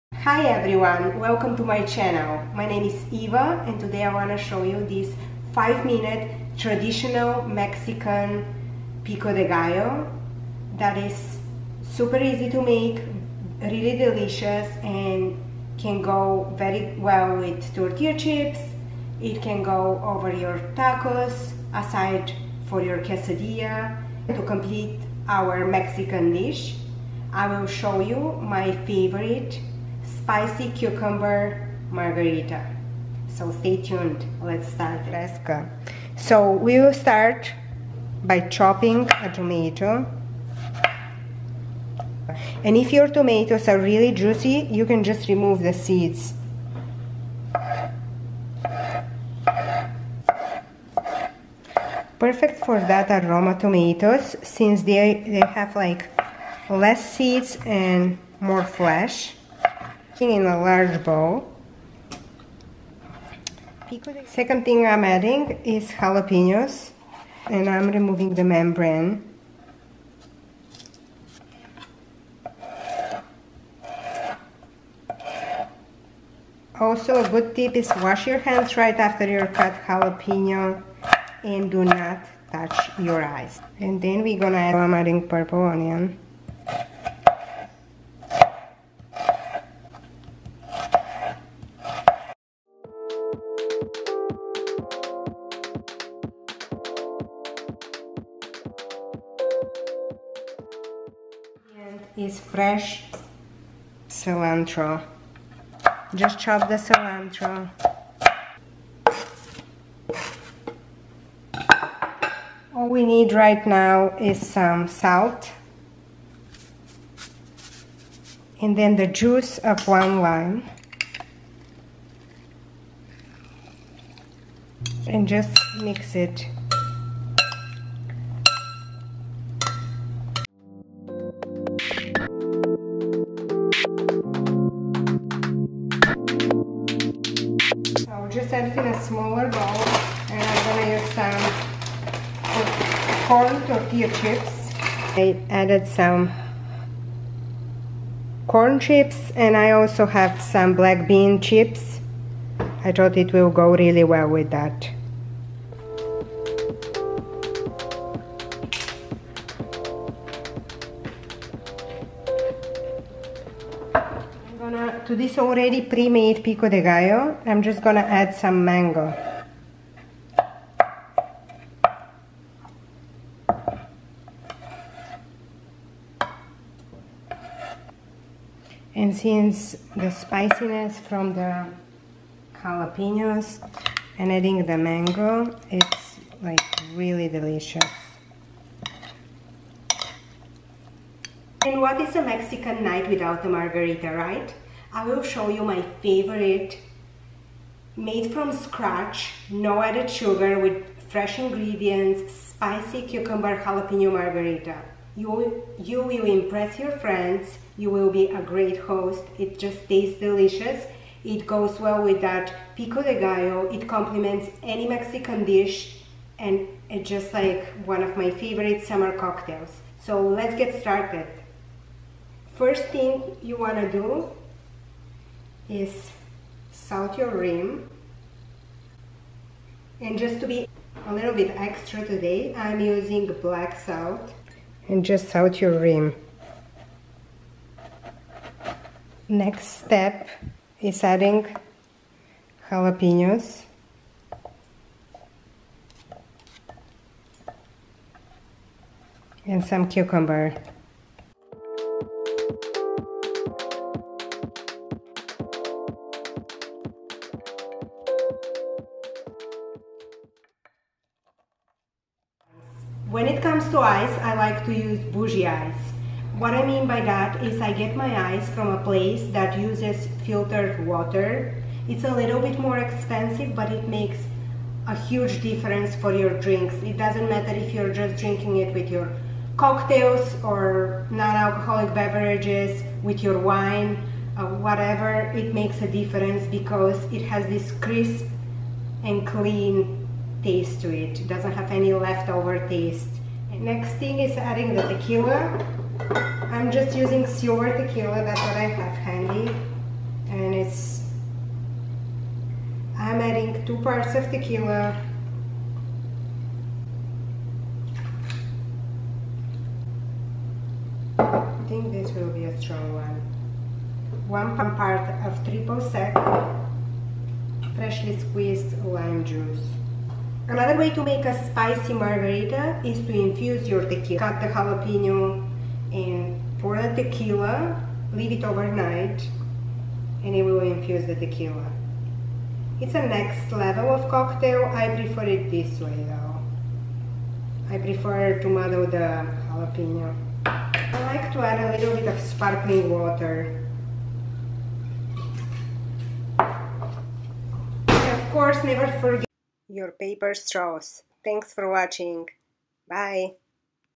PICO DE GALLO | How to make authentic Mexican salsa and the BEST SPICY MARGARITA
❤︎ Thumbs up if you enjoy this video ❤︎

Shop my closet on POSHMARK @ ivelinaangel